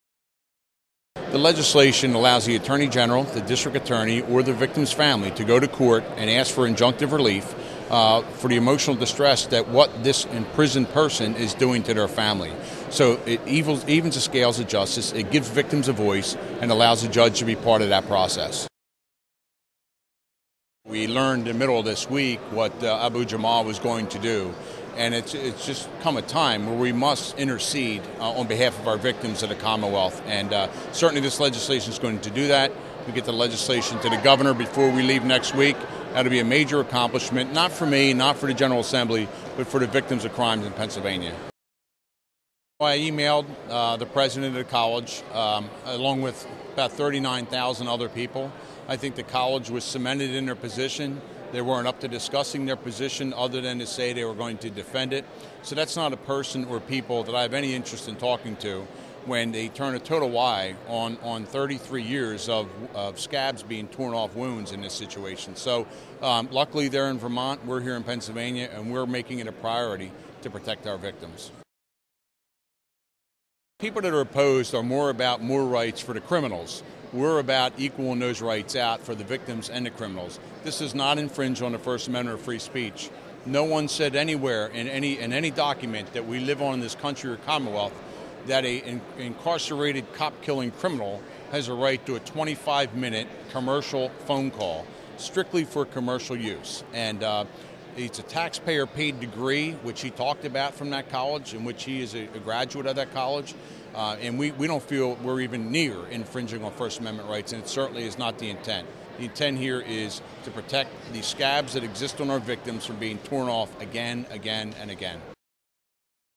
Revictimization Relief Act Protects the Victims
PA State Rep. Mike Vereb describes his bill to create the Revictimization Relief Act.